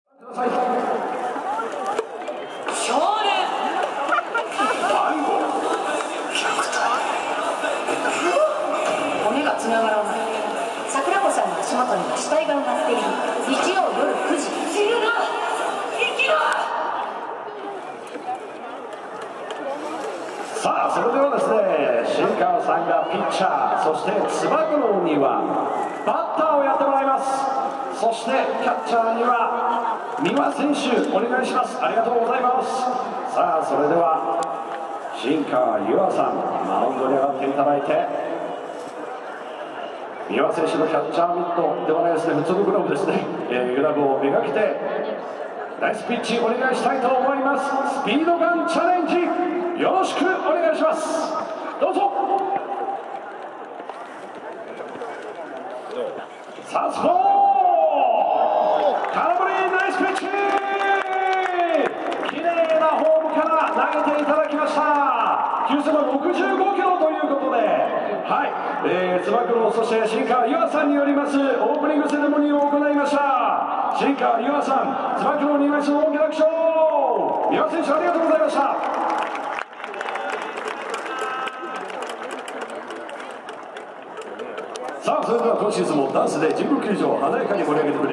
新川優愛 始球式 2017-4-28
新川優愛さんとつば九郎の始球式です。